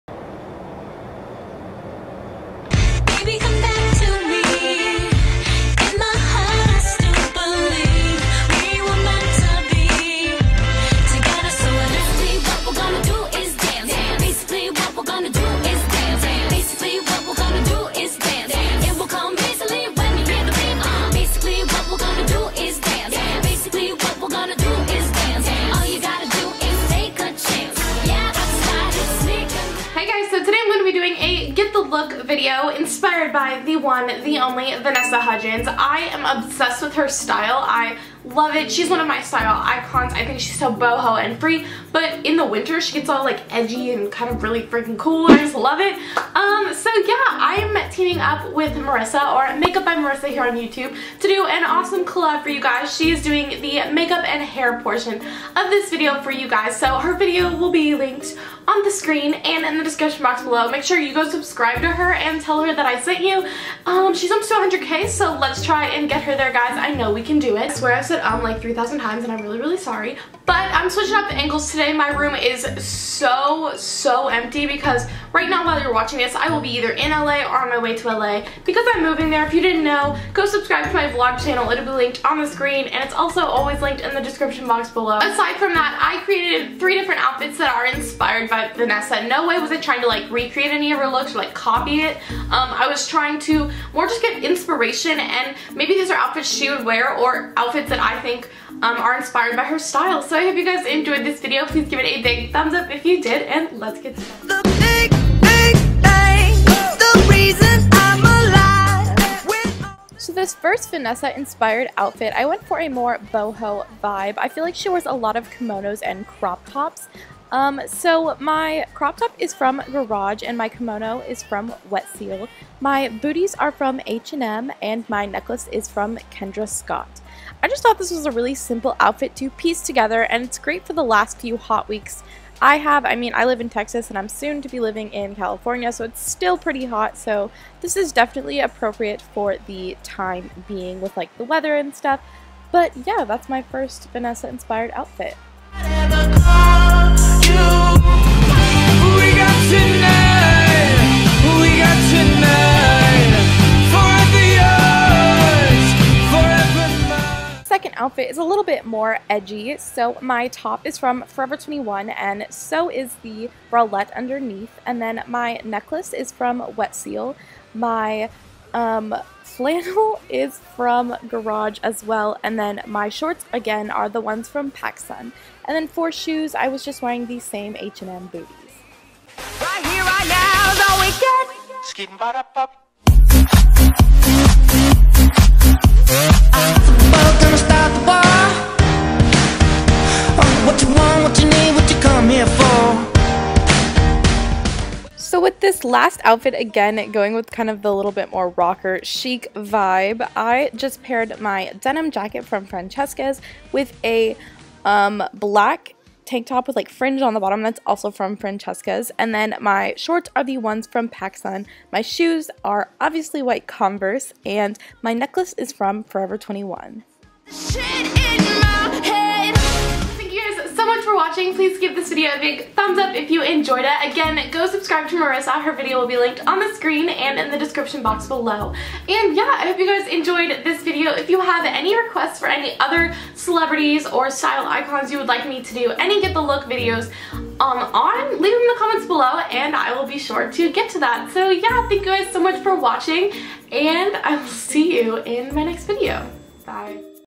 Vanessa Hudgens Inspired Outfits | Cicily Boone
In this video I show you how to put together 3 Vanessa Hudgens outfits that are easy to recreate with items in your closet! :)

Wanna be penpals...Send me something?! :)
Cicily Jo
Kyle, Texas 78640